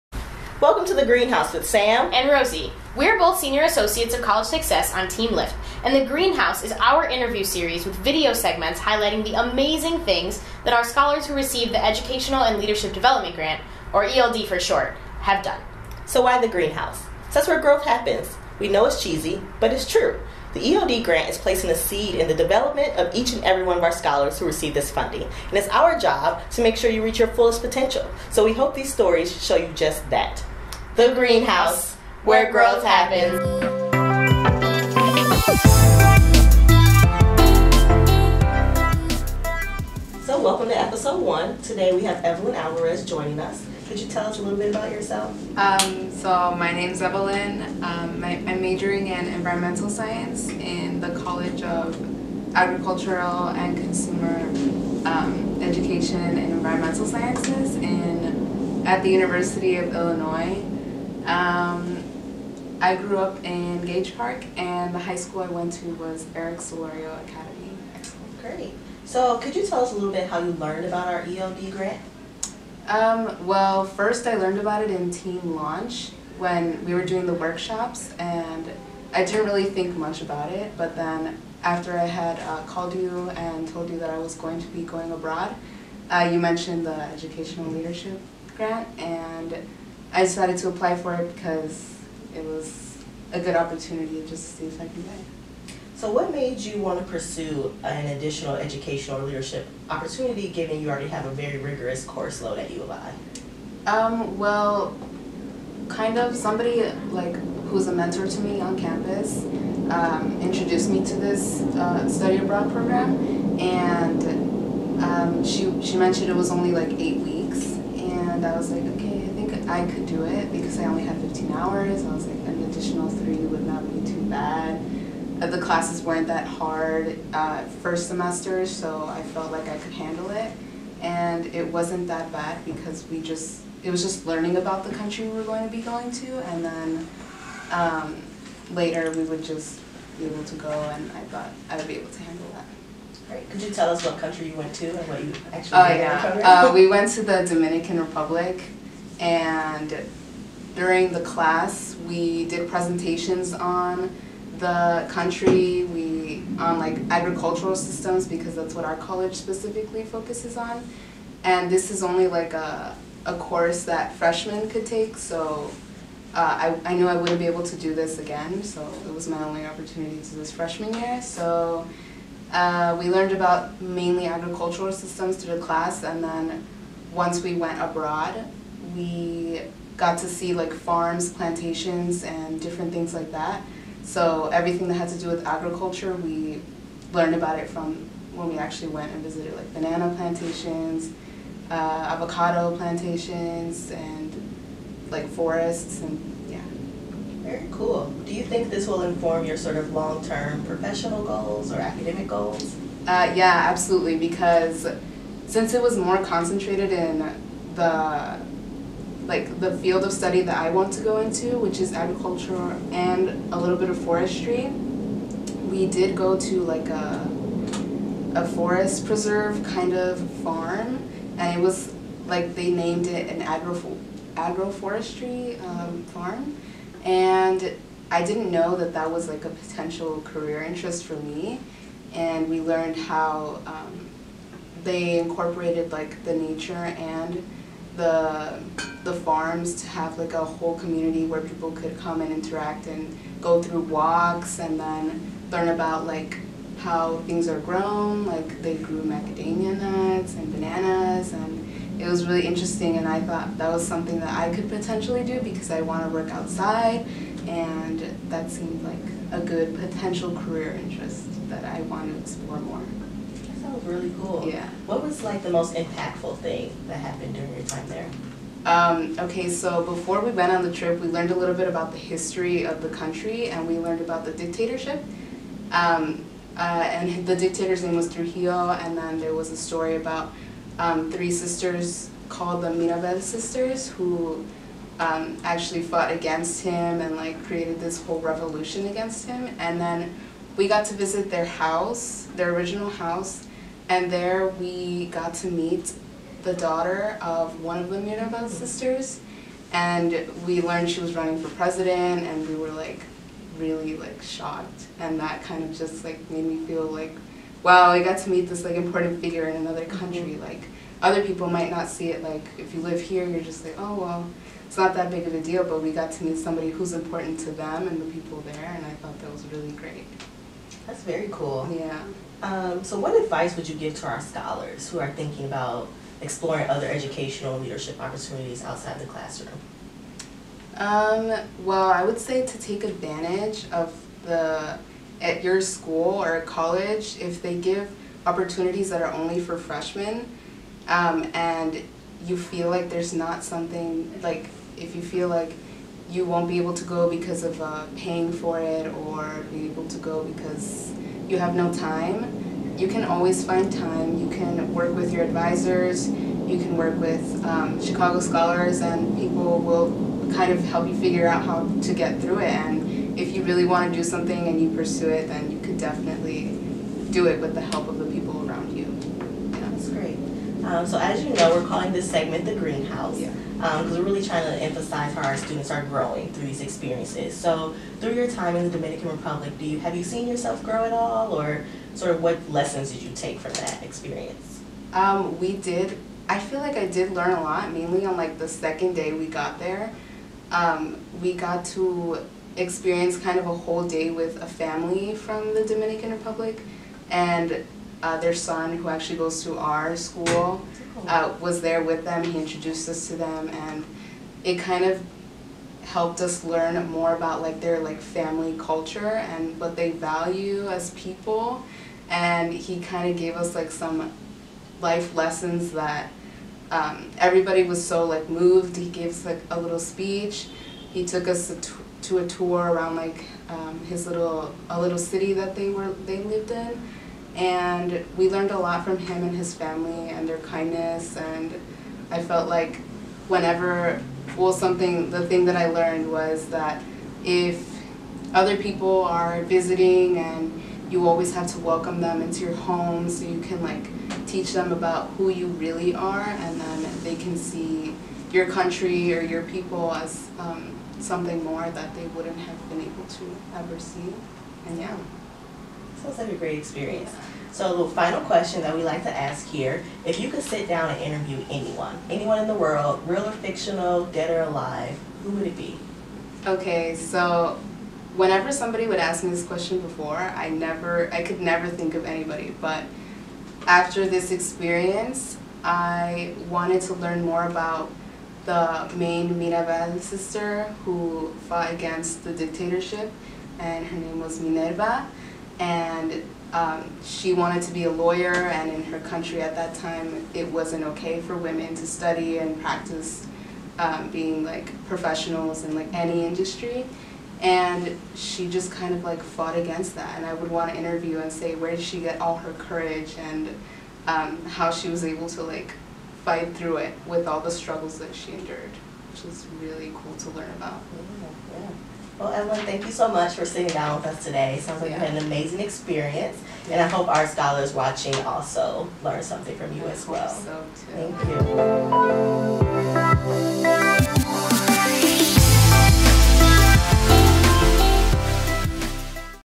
The Greenhouse Episode 1: Agricultural Systems in the Dominican Republic
In this episode we sit down with Evelyn Alvarez, a freshman at the University of Illinois at Urbana-Champaign, who spent a week discovering agricultural systems in the Dominican Republic.